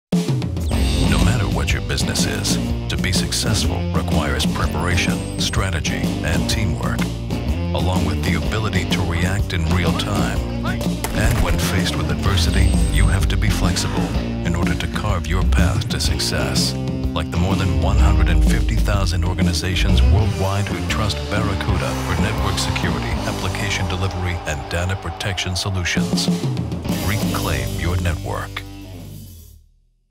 Football Spot :30 | Barracuda Networks
No matter what your business is, to be successful requires preparation, strategy and teamwork – along with the ability to react in real-time. When faced with adversity, you have to be flexible – in order to carve your path to success.

150,000 organizations worldwide trust Barracuda – for network security, application delivery, and data protection solutions.